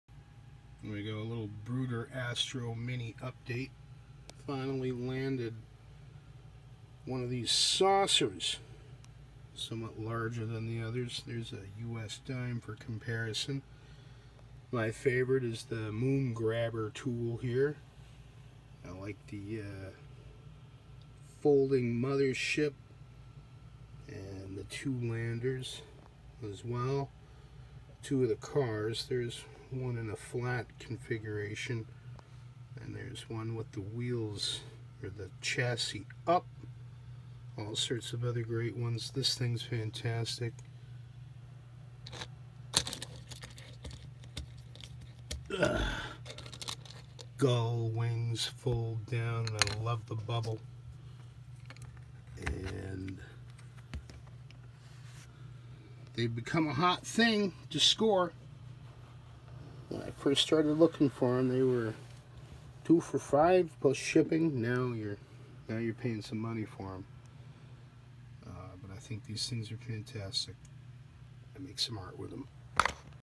Bruder "Astro Minis" Update: Snap-Together Space Plastic Made in West Germany
Now to snag that purple saucer. There's other Bruder space toy forms as well, very hard to find and cost a bundle. These have been relatively inexpensive to collect & make for fantastic diorama based art pix.